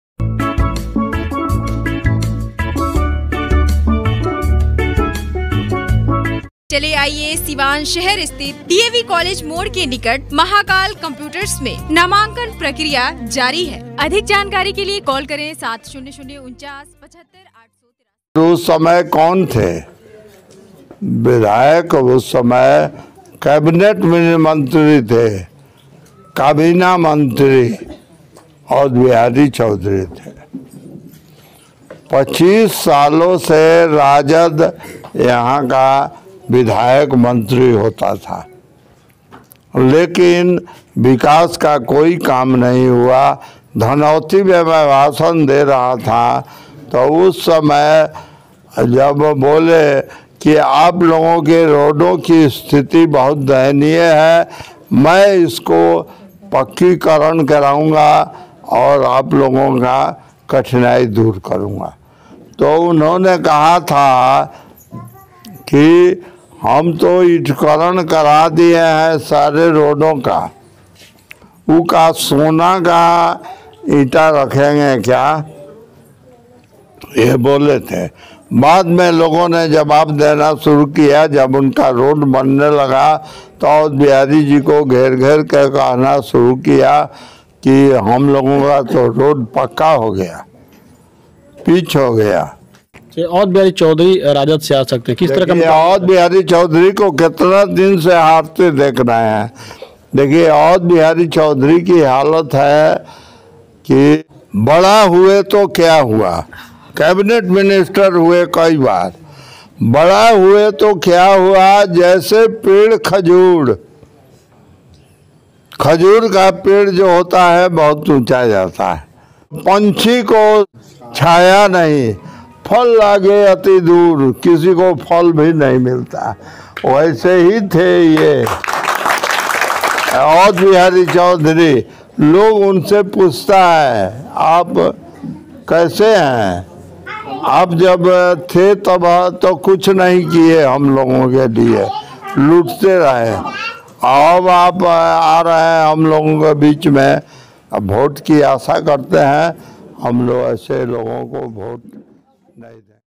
अवध बिहारी चौधरी पर व्यासदेव प्रसाद का बड़ा हमला, ये क्या कह दिया विधायक ने....
सिवान सदर भाजपा विधायक व्यासदेव प्रसाद ने राजद के पूर्व मंत्री अध्यक्ष चौधरी पर पड़ा हमला बोला है।